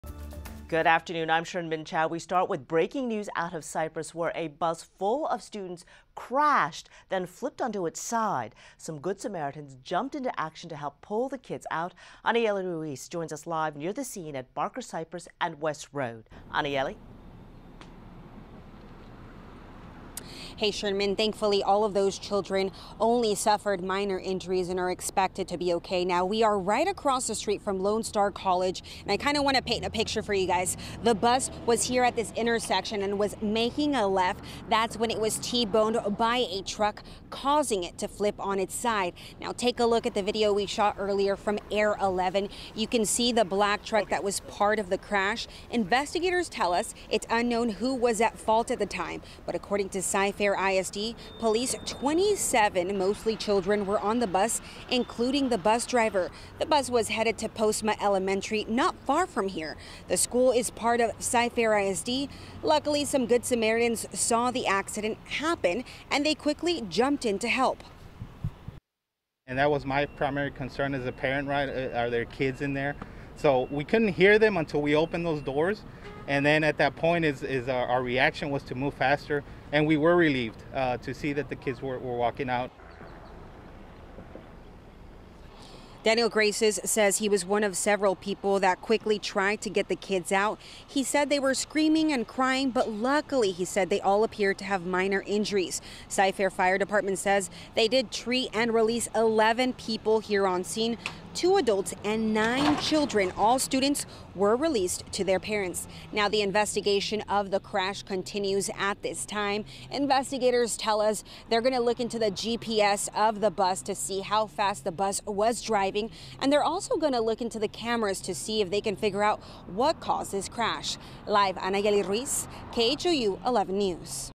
Cy-Fair ISD school bus filled with students flips to its side after being T-boned
A bus filled with students flips to its side after a crash involving a pick-up truck. Some Good Samaritans helped to rescue students off the bus.